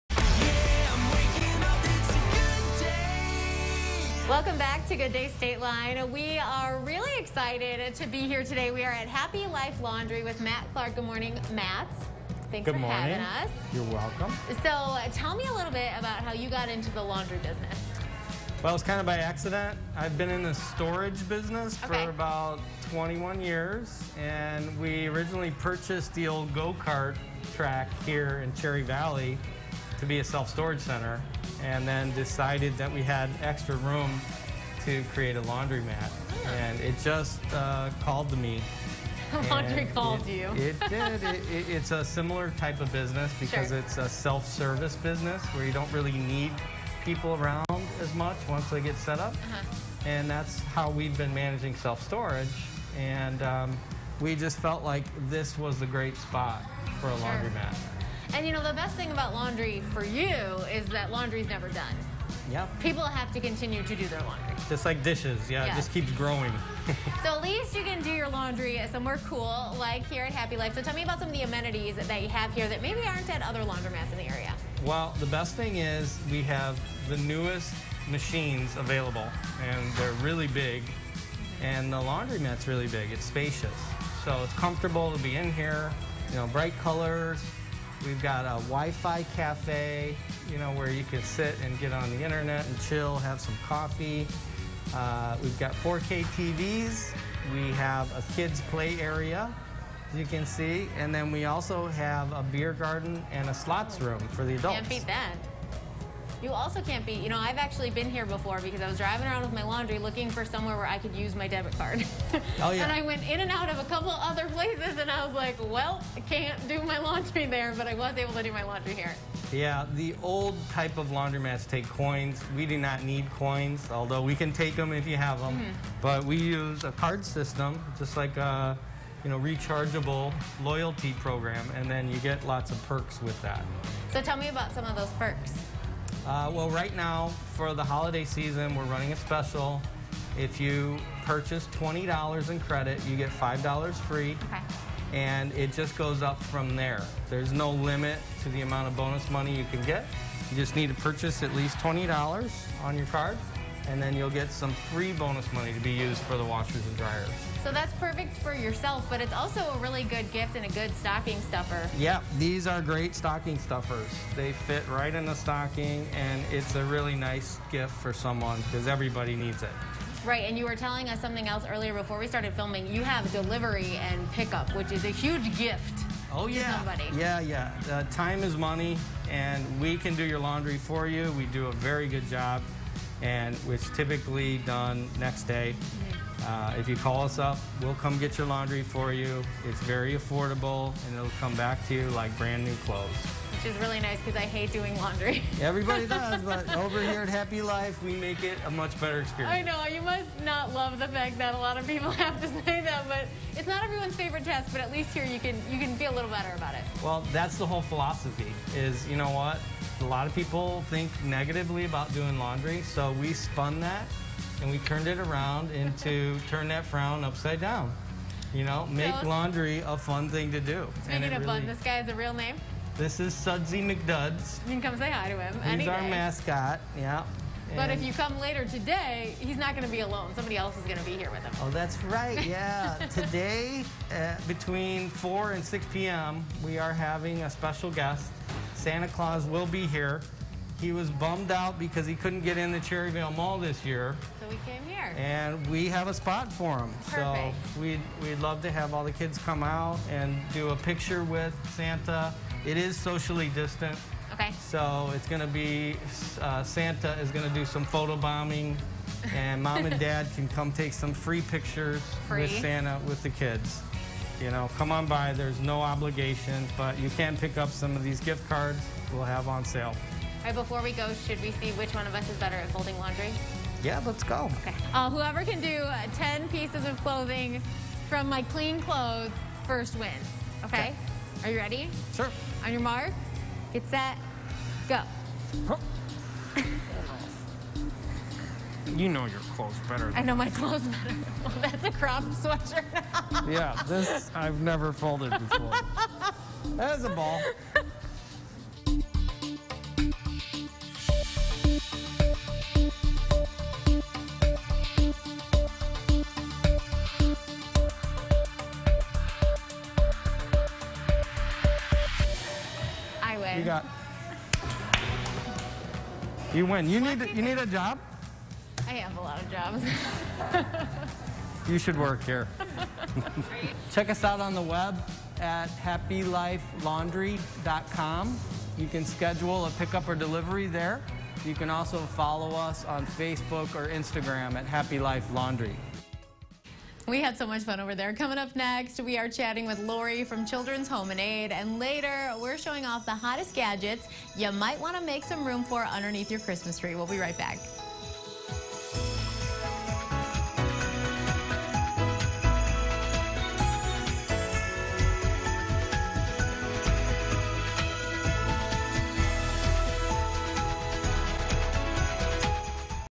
Happy Life Laundry has a holiday gift card special
No one likes doing laundry, but when you have TVs, slots, free wifi, and giant machines - it doesn't seem so bad! We went to Happy Life Laundry to check out their awesome facility and do some folding while we're there! They have a holiday gift card special right now where you can get extra bucks for everything you spend. Happy Life Laundry is located at 7215 Cherryvale N Blvd (right next to the mall). You can find more information at happylifelaundry.com or follow them on Facebook @happyliferkfd.